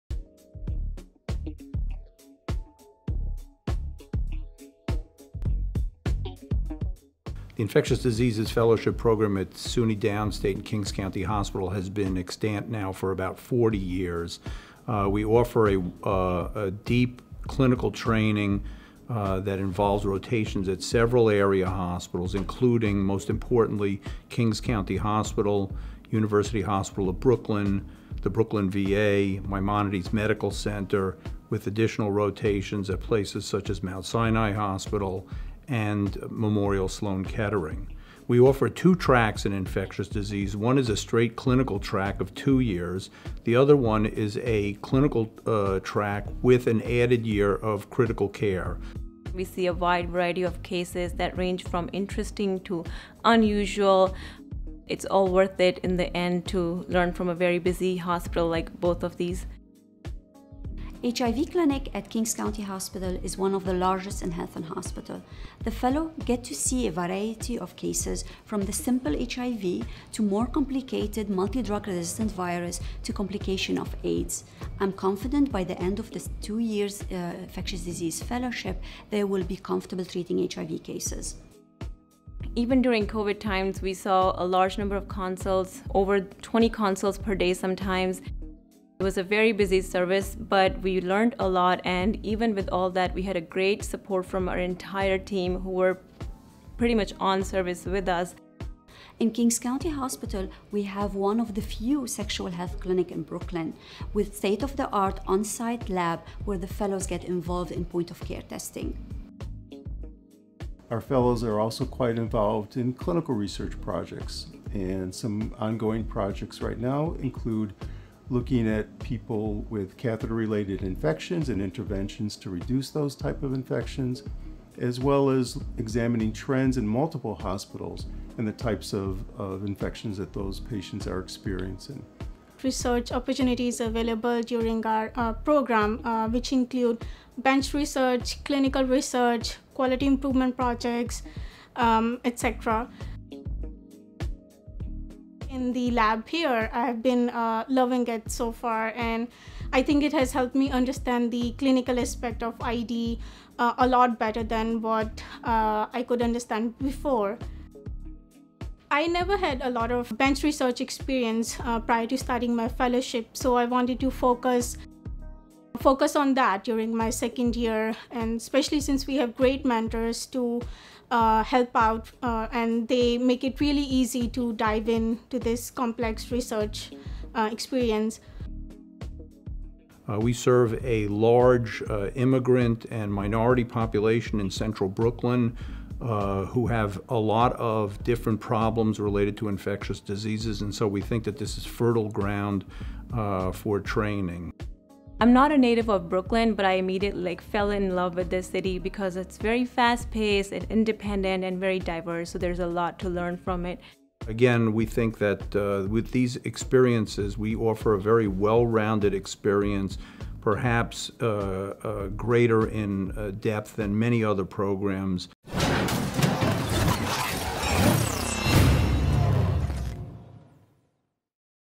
SUNY Downstate Division of Infectious Diseases
The Division of Infectious Diseases Medicine at SUNY Downstate Medical Center is now and has been over the last few decades at the forefront of many of the most exciting challenges in Medicine. From HIV research and care to enigmas of tropical medicine and transplant immunosuppression and the daunting problems posed by multidrug-resistant pathogens our team of clinicians, educators and researchers have had the opportunity to learn about and manage an astonishing range of pathologic conditions. Our busy campus located in the middle of Brooklyn is inarguably one of the finest places to see and take care of patients with a host of infectious diseases problems unlikely to be seen at many other institutions. Close knit support from our superbly trained colleagues in pharmacy and clinical microbiology is outstanding and enable the division to function at the highest level.

We operate several multi-specialty clinics including the largest NYC-sponsored HIV Care Center. These clinics have a wide range of ancillary support services including resources for LGBTQ patients, those with substance abuse issues, and those coinfected with hepatitis C and B. Additionally we operate the only non NYS sponsored STI Clinic equipped with on-site stat lab facilities and rapid access to directly delivered medications.

In sum, the SUNY Downstate Infectious Diseases Section offers unparalleled opportunities for patient care, training and research.